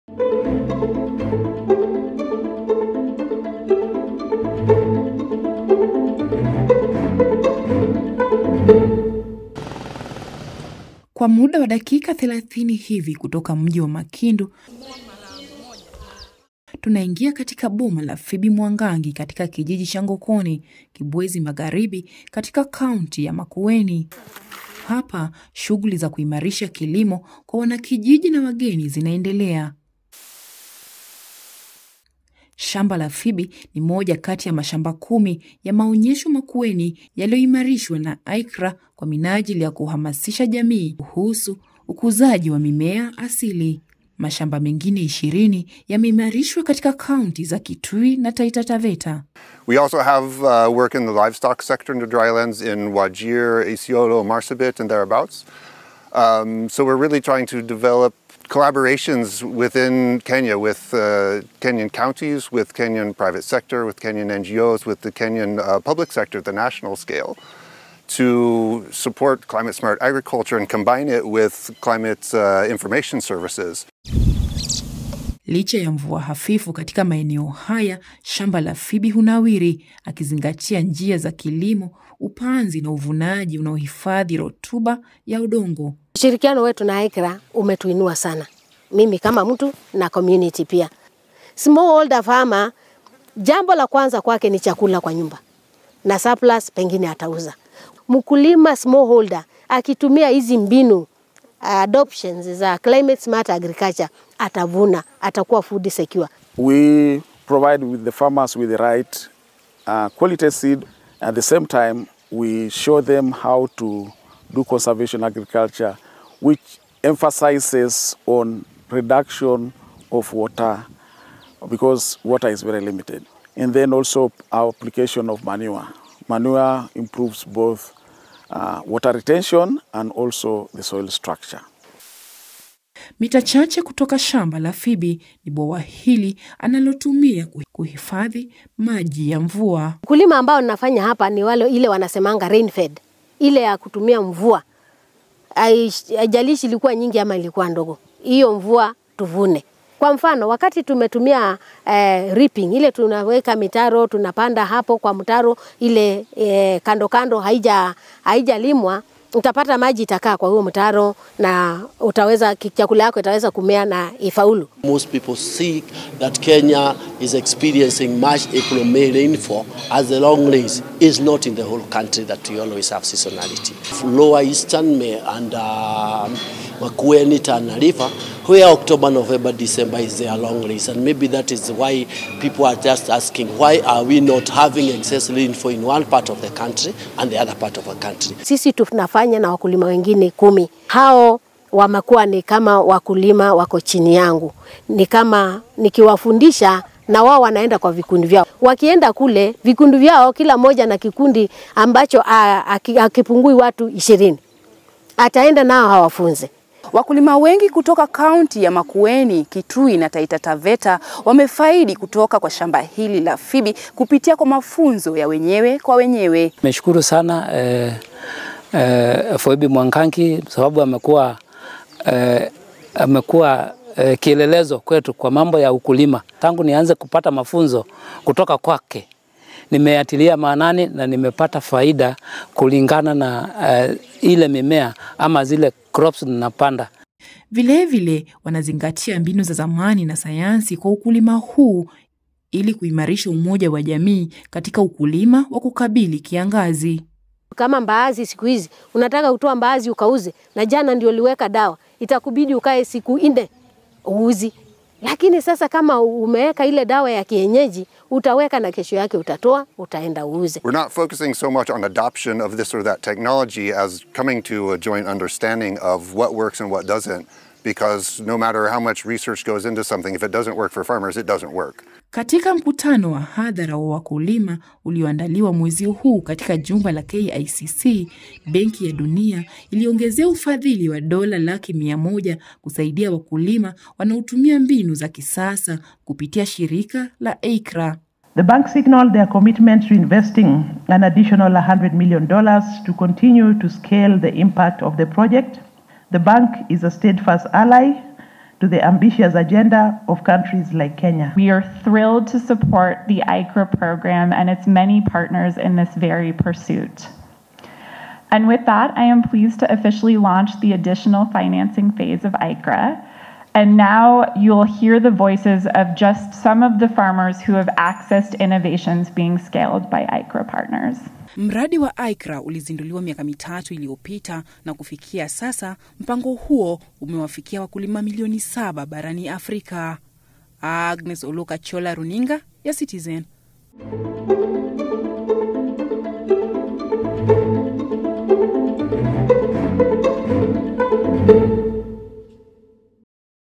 Kampeni ya kilimo cha kustahimili ukame yaendeshwa katika kaunti za Makueni, Kitui na Taita Taveta
Kaunti za Makueni,Kitui and Taita Taveta ni baadhi ya maeneo ambayo hupata mvua kidogo na kukabiliwa na changamoto za chakula. Juhudi sasa zikifanywa kuhakikisha kilimo chenye uwezo wa kustahimili ukame. Kampeni hii ikiendeshwa kwa ushirikiano wa Shirika la AICCRA na washika dau wengine. Agnes Oloo alikuwa makueni na kuandaa taarifa ifuatayo.